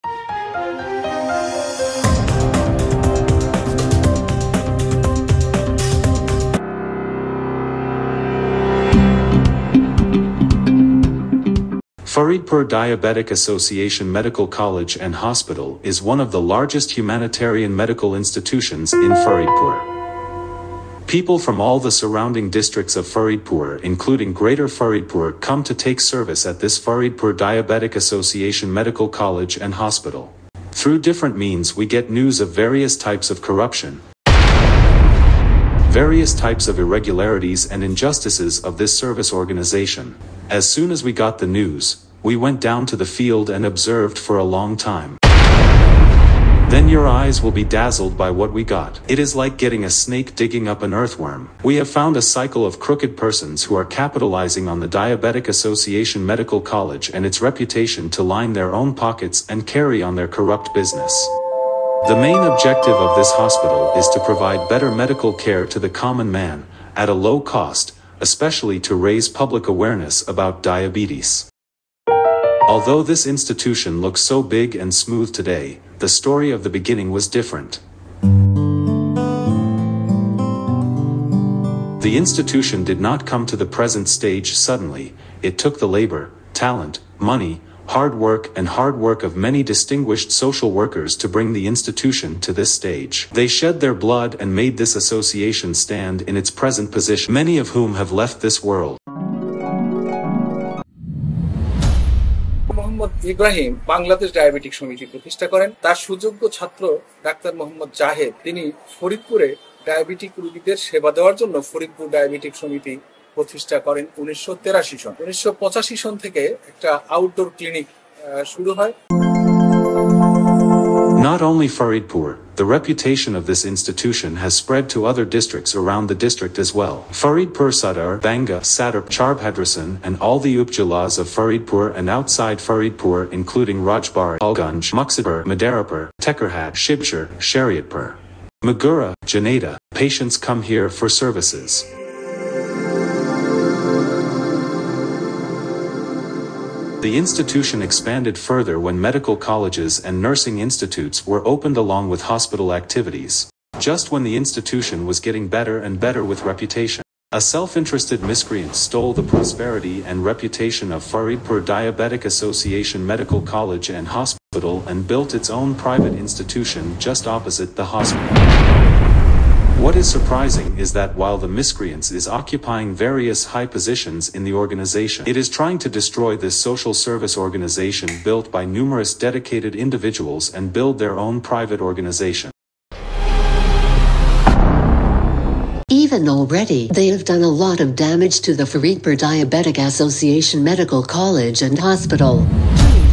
Corruption at Diabetic Association Medical College and Hospital
Faridpur Diabetic Association Medical College and Hospital is one of the largest humanitarian medical institutions in Faridpur. People from all the surrounding districts of Faridpur including Greater Faridpur come to take service at this Faridpur Diabetic Association Medical College and Hospital.

Through different means, we get news of various types of corruption, various types of irregularities, and injustices of this service organization. As soon as we got the news, we went down to the field and observed for a long time. Then your eyes will be dazzled by what we got.

It is like getting a snake digging up an earthworm. We have found a cycle of crooked persons who are capitalizing on the Diabetic Association Medical College and its reputation to line their own pockets and carry on their corrupt business.

The main objective of this Hospital is to provide better medical care to the common man, at a low cost, especially to raise public awareness about diabetes. Although this institution looks so big and smooth today, the story of the beginning was different.

The institution did not come to the present stage suddenly, it took the labor, talent, money, hard work, and hard work of many distinguished social workers to bring the institution to this stage. They shed their blood and made this association stand in its present position. Many of whom have left this world.